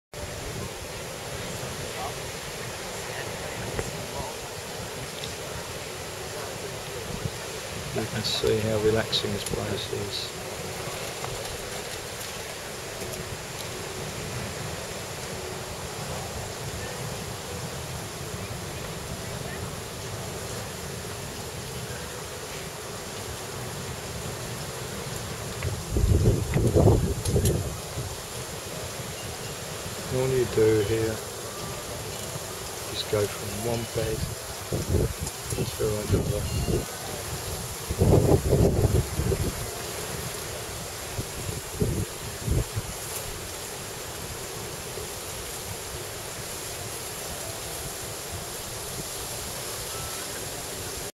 Maroma Resort Cancun by the Pool at Midday.
People chilling by the pool around Midday